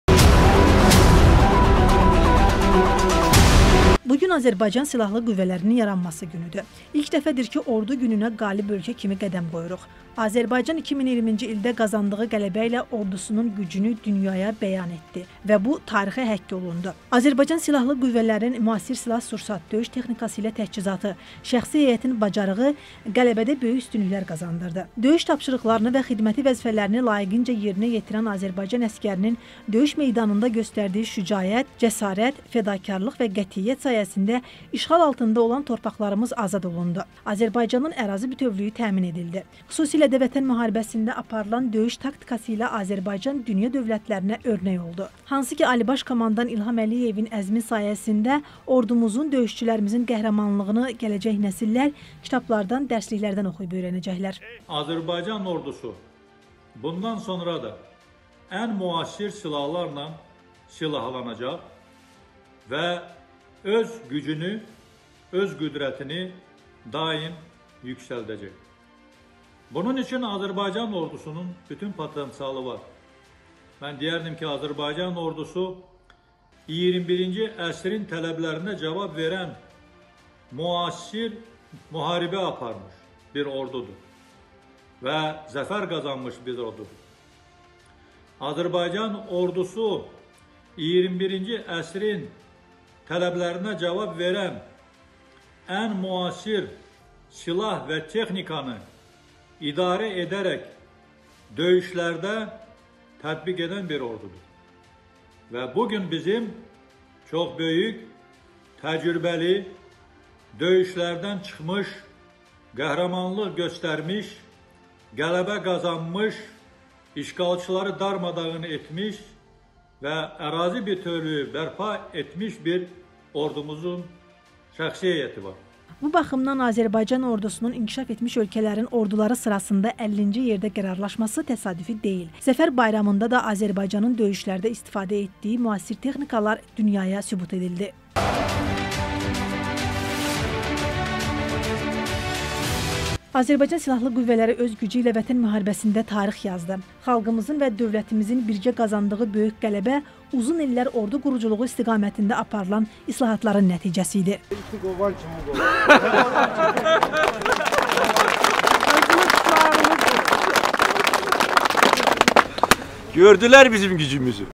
Azərbaycan Ordusunun gücü
Silahlı Qüvvələr Gününə qalib ölkə kimi qədəm qoyuruq